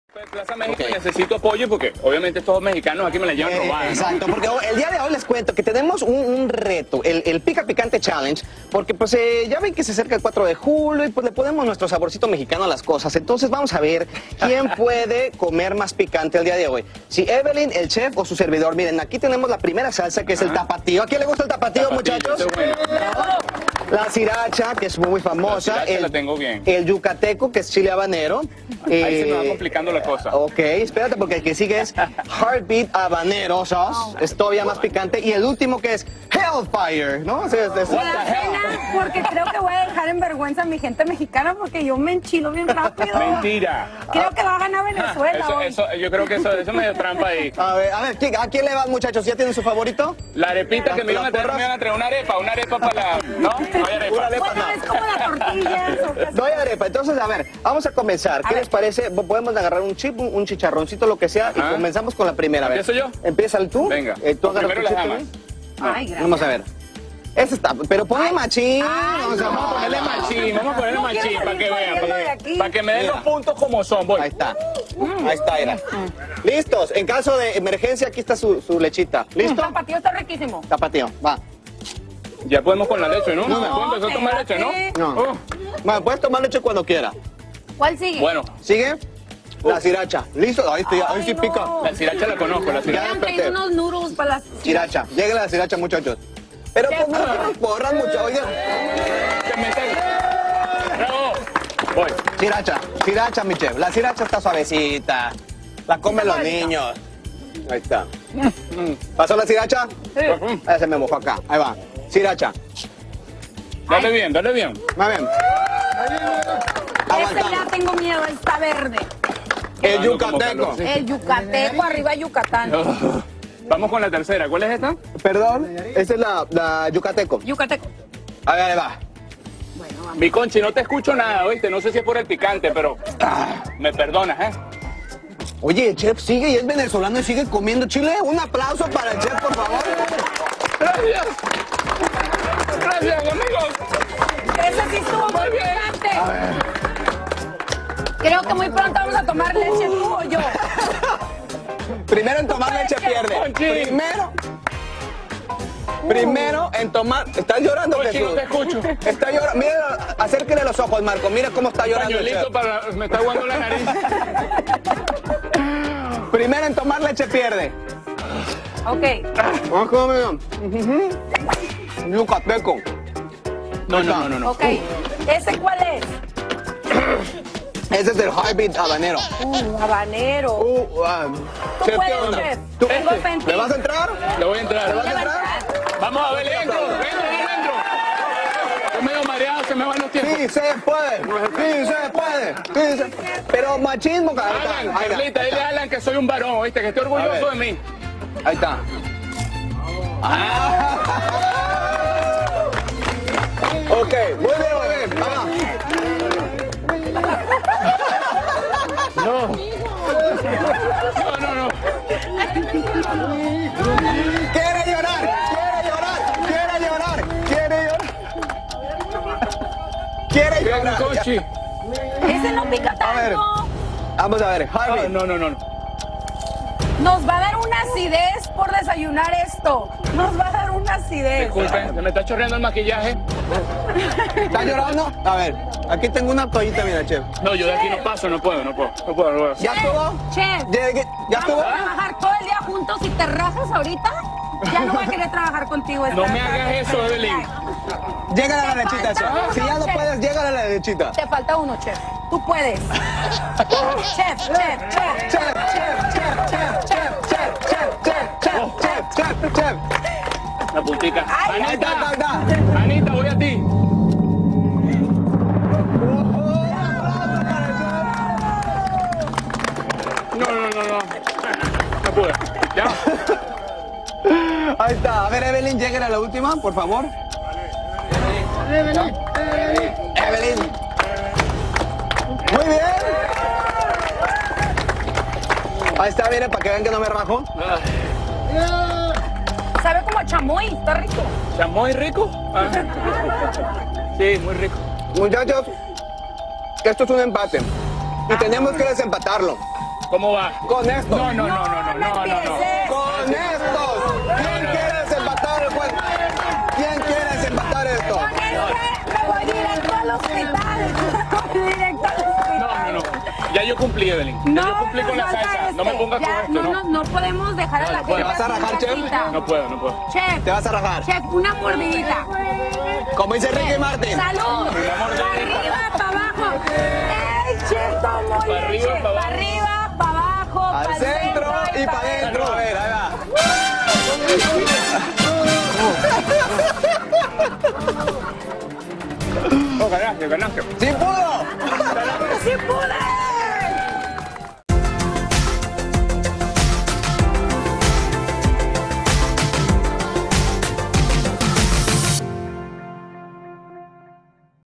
El chef Yisus suda y llora en este reto de probar chiles muy picantes
Luis Sandoval retó al chef Yisus y a Evelyn Sicairos a cumplir valientemente este 'Picante challenge'. Y aunque ninguno de los tres se echó para atrás, sí vimos una que otra lagrimita provocada por el intenso sabor de estas salsa y chiles.